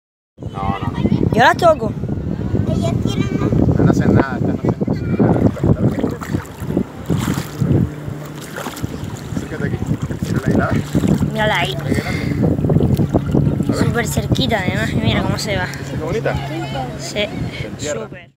Las rayas mantelinas 'vuelven' de visita a Salinetas
La playa de Salinetas recibió este sábado la visita de varios ejemplares de rayas mantelinas (Gymnura altavela), asiduas a esta cala teldense. Ante la mirada atónica de los bañistas menos acostumbrados a su presencia, se pasearon por la orilla, despertando la curiosidad de los más pequeños